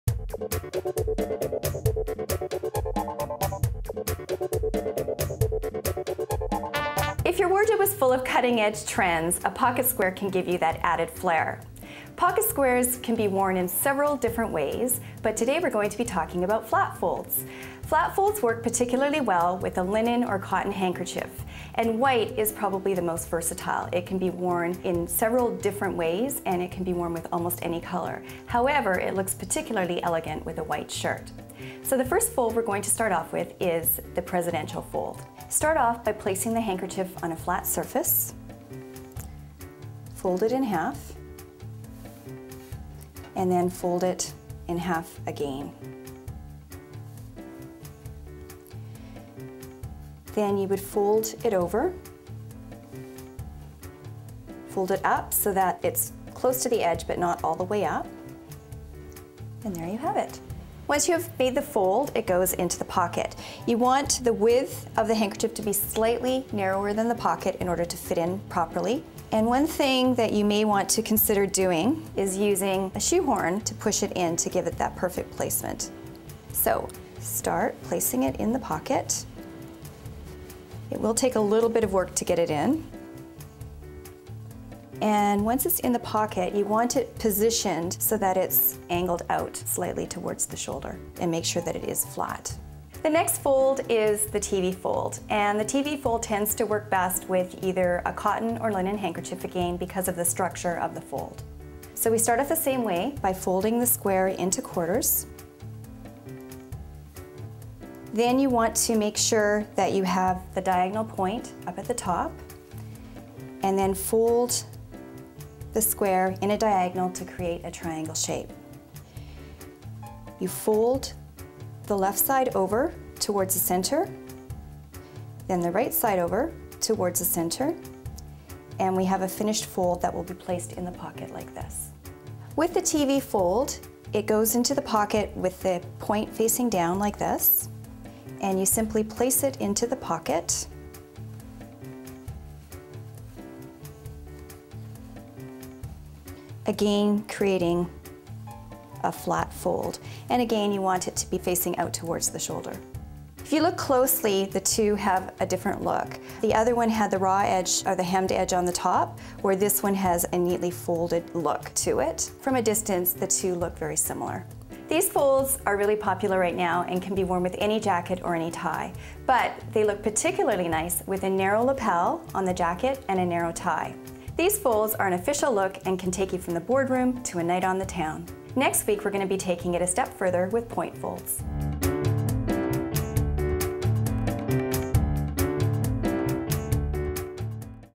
How to fold pocket squares - Flat Folds
Learn how to fold pocket squares in flat folds with personal image consultant Kimberly Law.